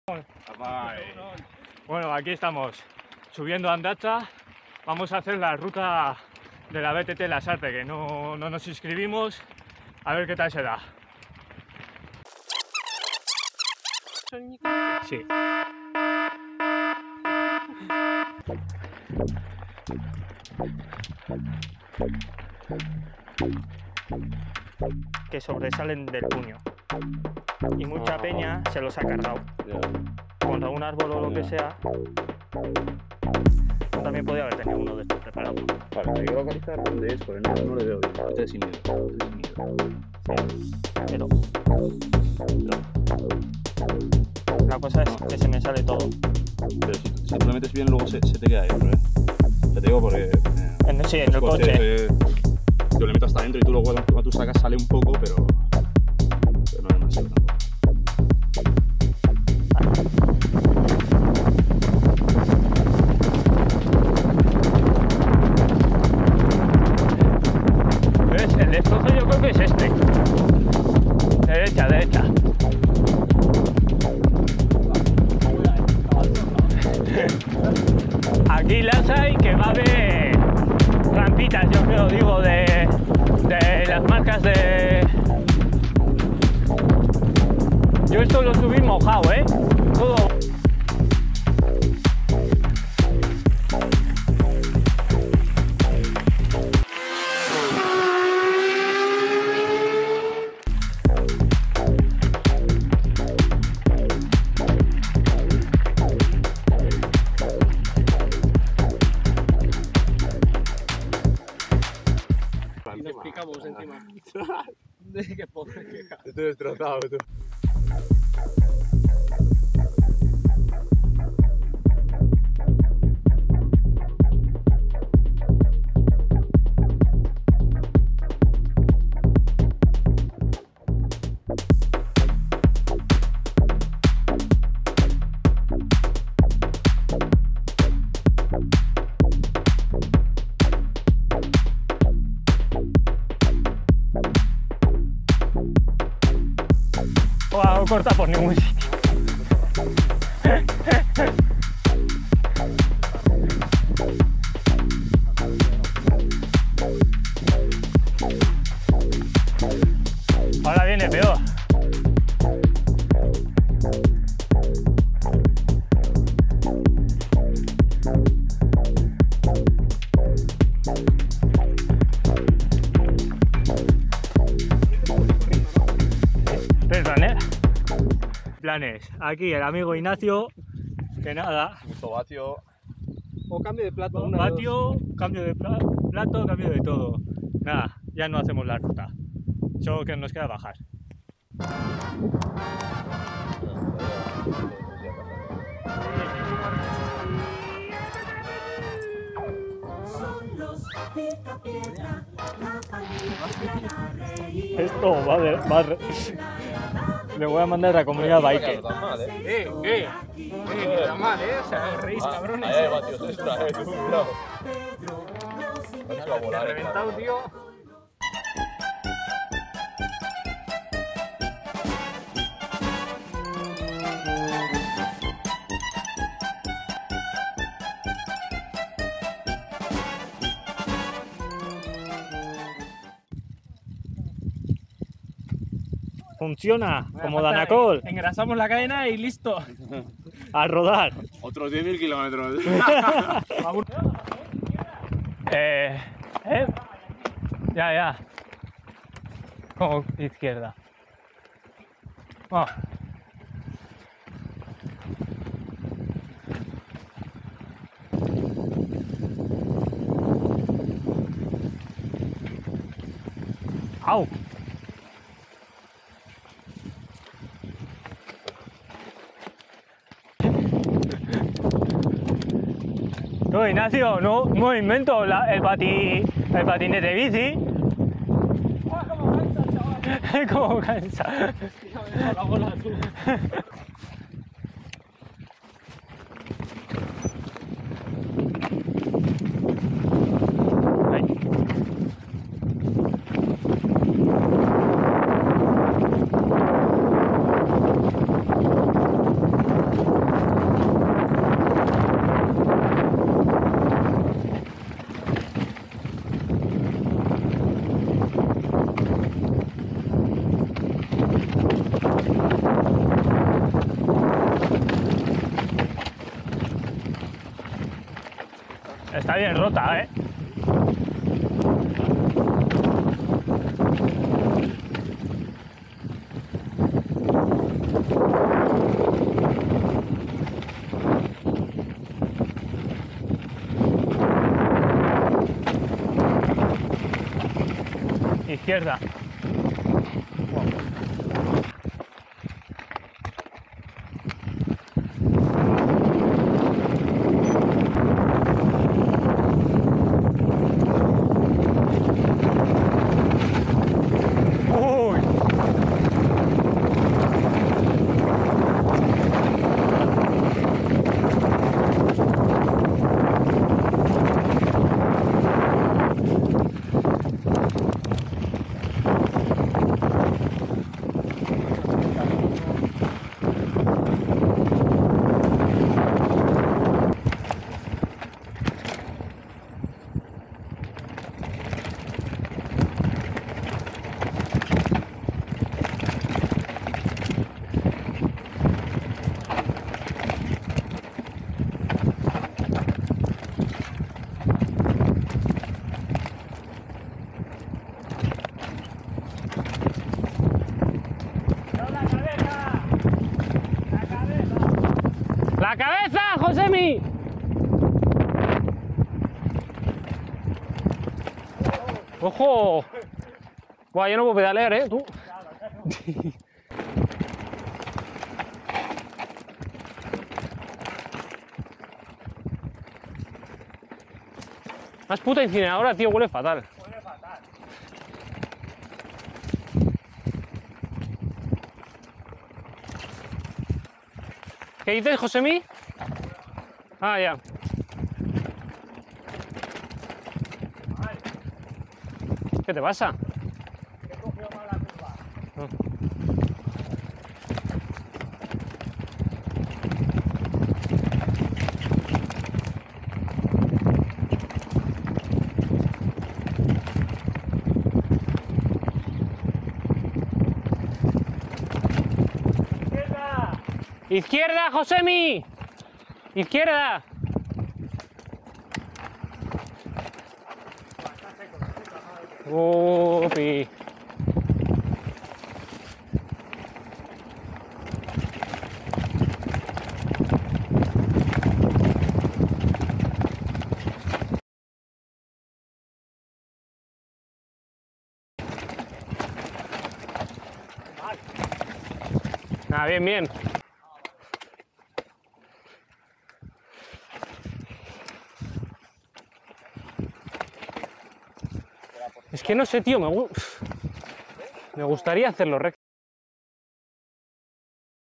Lasarte-Oria BTT. ¡todo mal! (a nuestra bola) | BTT-MTB
¿A alguien o a algún conocid@ le ha pasado algo parecido?

[CAS] Con la intención de hacer la ruta larga de la marcha Lasarte-Oria BTT, pero avería gorda del amigo, chapuza para sobrevivir y al pintxo.

[EUS] Gaurkoan Lasarte-Oria BTT martxako ibilbide luzea egiteko intentzioarekin, baina lagunaren matxura larria, beraz tsapuza bat egin bizirik irauteko eta pintxora.